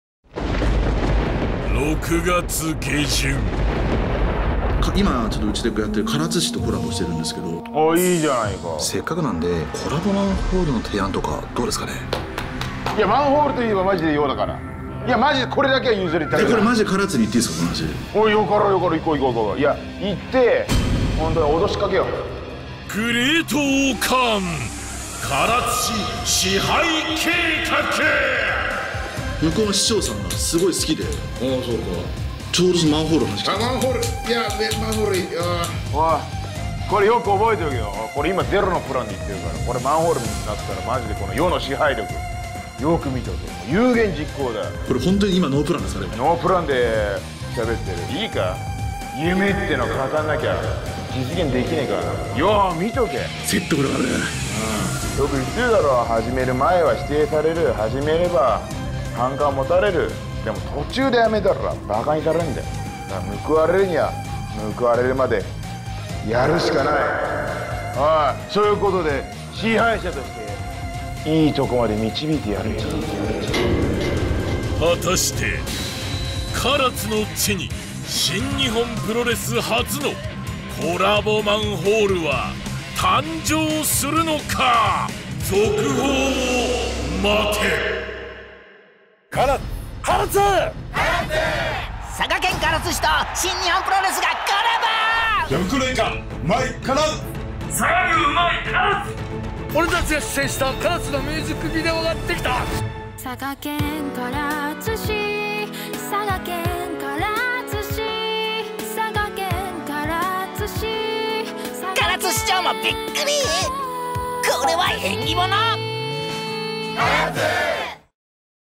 オイ！コラボマンホール作れ！グレート-O-カーンの唐津市支配計画！
プロレス界一のマンホールフリーク、グレート-O-カーンが
唐津市の支配に動き出す！
果たしてコラボマンホールは実現するのか！？続報を待て！